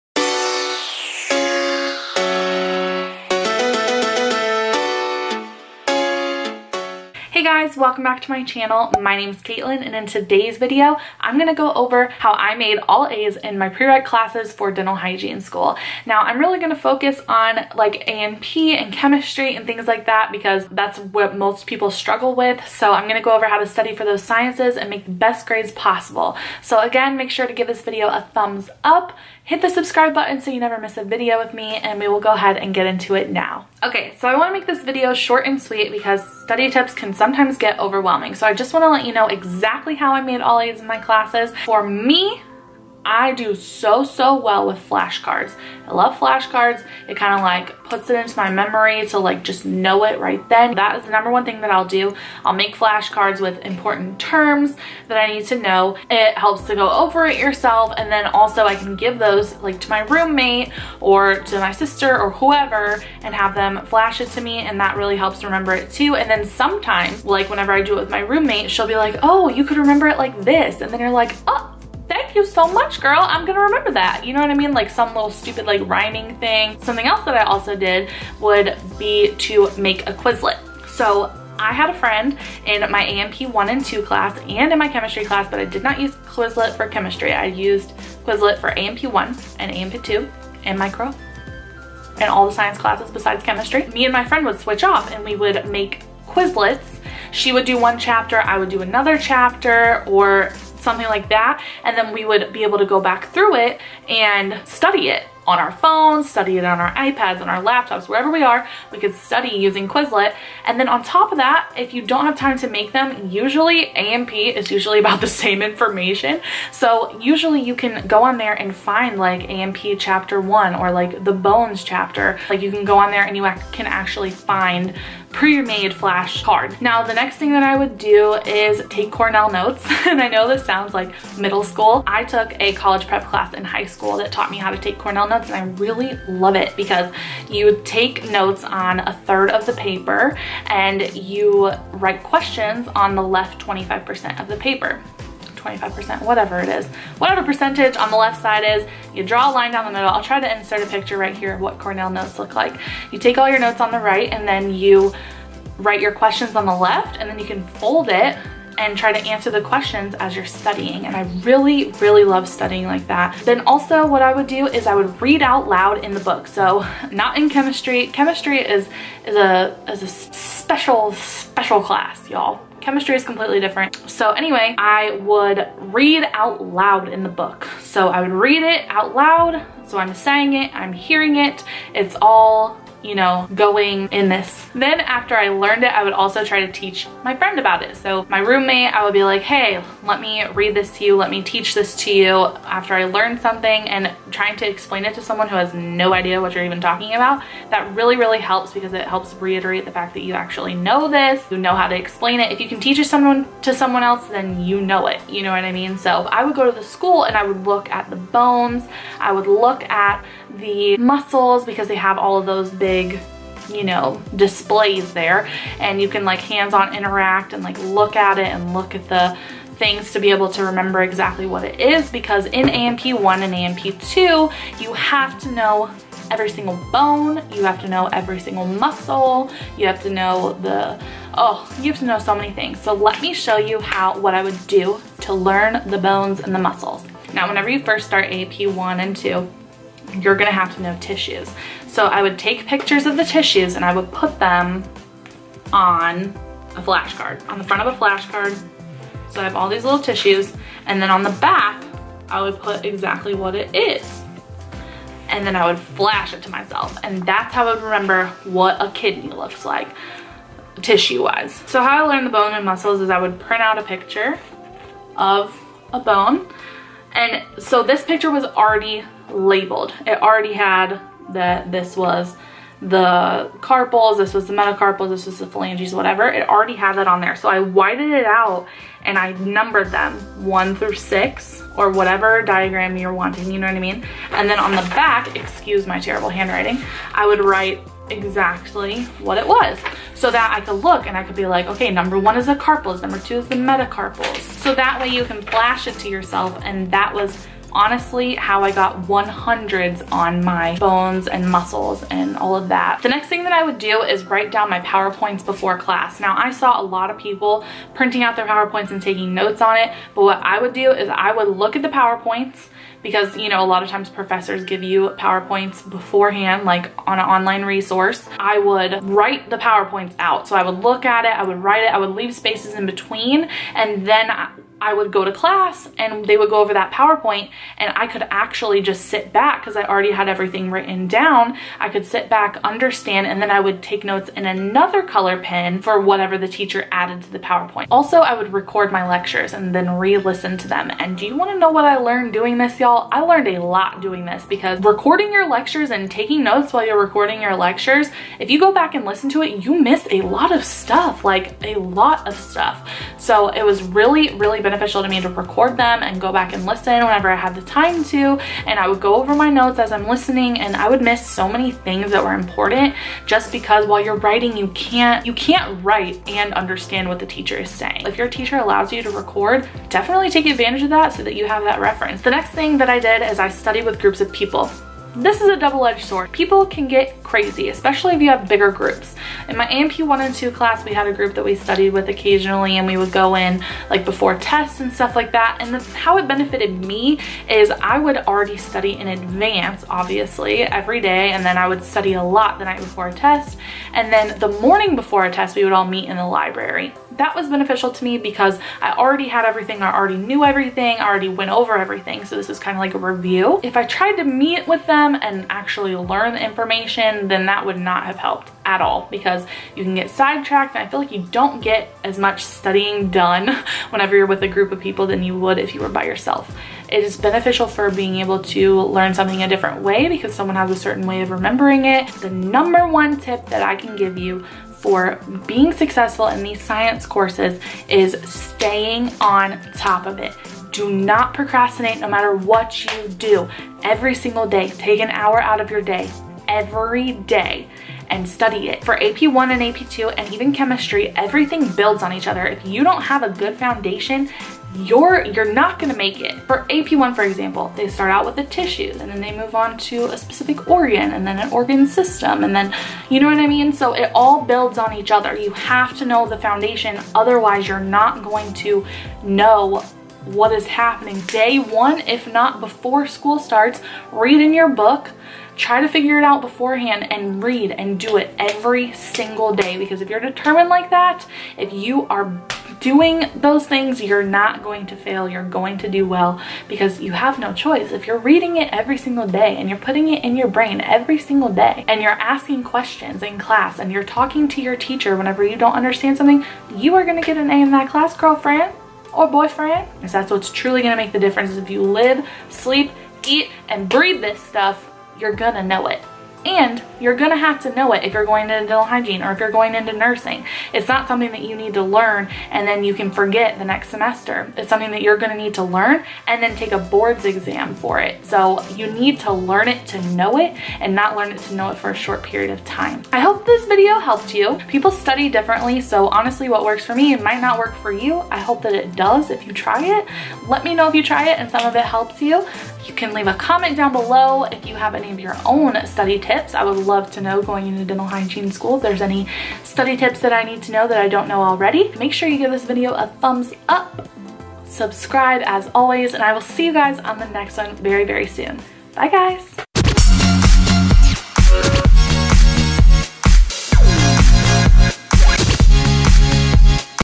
MAKE ALL A'S IN SCIENCE PRE-REQ'S | COLLEGE STUDY TIPS | DENTAL HYGIENE SCHOOL
Hey guys! In this video I'm giving all of the details on how I studied and made all A's in my science prereq classes. I hope you enjoy!!